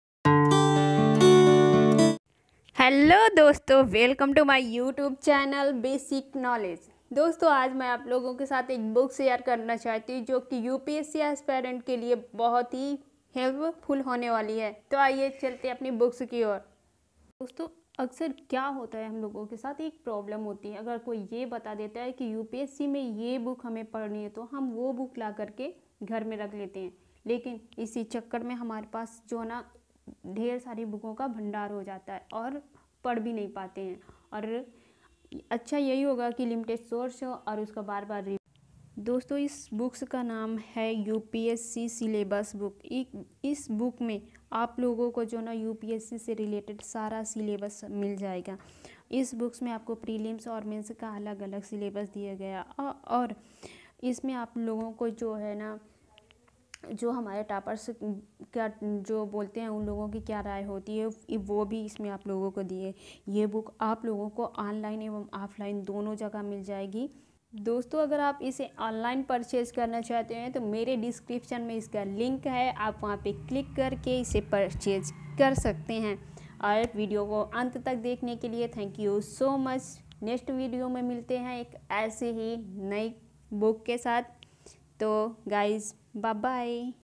upsc new syllabus book ,2019 के साथ आईएएस प्रारंभिक और मुख्य परीक्षा को क्रैक करने के tips
upsc new syllabus book,upscbook upsc knowledge
यूपीएससी के नए पाठ्यक्रम और रैपिड जीके 2019 के साथ आईएएस प्रारंभिक और मुख्य परीक्षा को क्रैक करने के टिप्स
स्पेक्ट्रम द्वारा आधुनिक भारत का इतिहास 2019-20 संस्करण (आधुनिक भारत का संक्षिप्त इतिहास) (हिंदी)